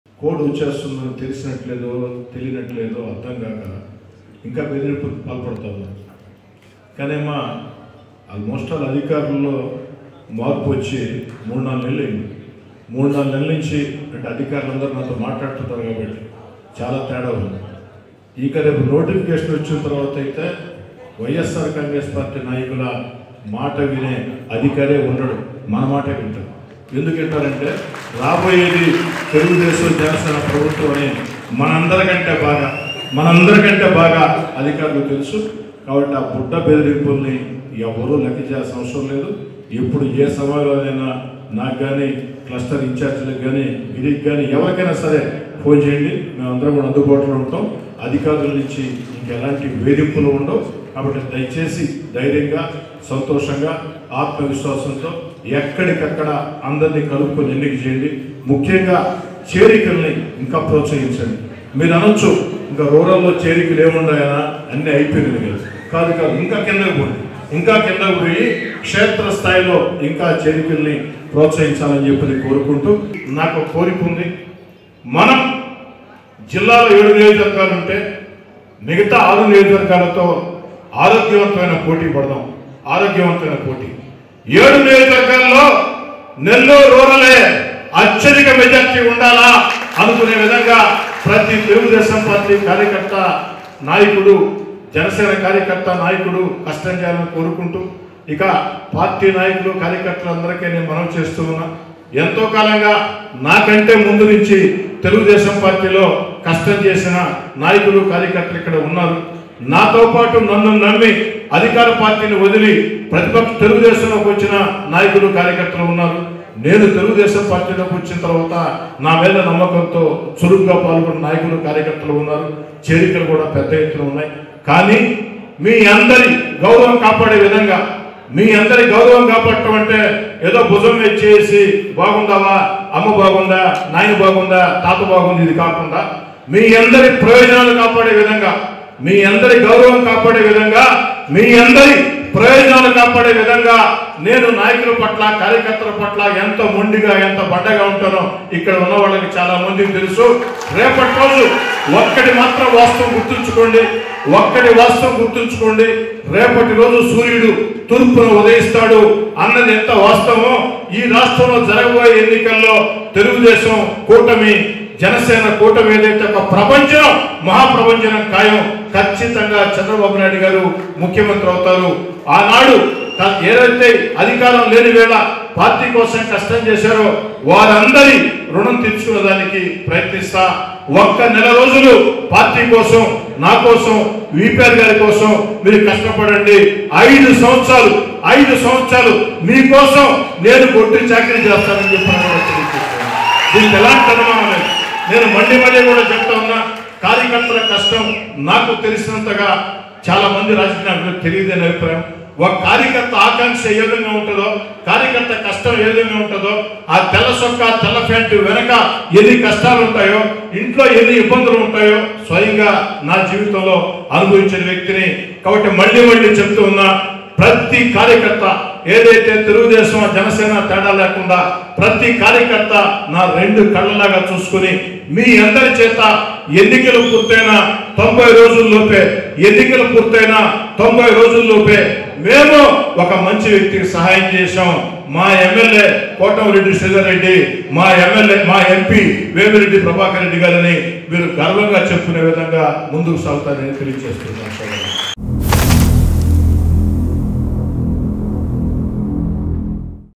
నెల్లూరు రూరల్లో సమరానికి సై.. వైసీపీతో యుద్ధం చేద్దాం.. క్యాడర్ తో కోటంరెడ్డి | Voice5
నెల్లూరు రూరల్లో సమరానికి సై.. వైసీపీతో యుద్ధం చేద్దాం.. క్యాడర్ తో కోటంరెడ్డి